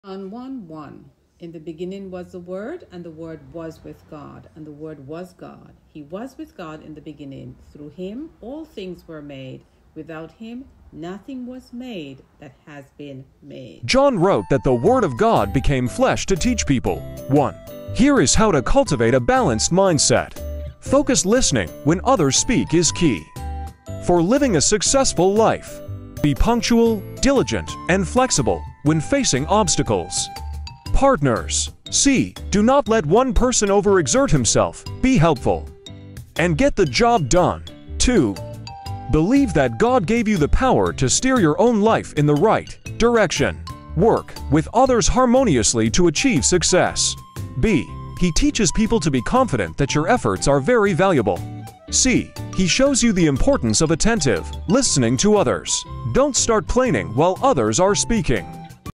Help Is On The Way To You BeMoreLightHearted ChallengesAreASpringboard for personal growth!
Jesus teaches the people how to cultivateAMentalllyBalancedApproach to life, especially when facing obstacles. We are to not just exist in the world but take time to enjoy it and learn how to do good.

Throughout TheBookOFJohn He teaches us how to take a moment and participate in the work that other people are doing. He helps us to see how important it is to be speaking words that can upliftothers and build confidence in them. Although in today’s fast-paced societies sometimes it’s easy to be caught up in our own lives and forget the importance of contributing positively to the lives of others. The main purpose to life is having a willing heart to share yourself with other people. Even if it is just to listen to them.

(Genesis1:1-28 and John 1:1-3) is helping us to see the value of sharing responsibility, this passage along with others teaches that God did not slave himself to prepare the world; He worked alongside others. By using encouraging words and working as a team together, with others towards a common goal. It enables us to see; effective, sustainable community led action, and the great rewards possible when your goals are accomplished.

Believe the word of God!
As we learn how to cultivate a positive environment in our own communities; without too much prior notice, it will foster growth and faster development and a more peaceful life for each individual. Sometimes some of the most rewarding experiences comes to me when we work as a team to feed hungry people, it emphasizes community-ownership and prevention of starvation.

God gave you the PowerToSteerYourLife in the right direction by WorkingHarmoniously with others to achieve success. Let’s strive to be more mindful of the words that we are speaking and make a bigger effort to be more supportive and helping one another to turn our life into something better where everyone can thrive.

Today’s clip provides you 3 keys to living out God’s plan for your life and the value of participation in the work that others are doing to grow more businesses and better relationships!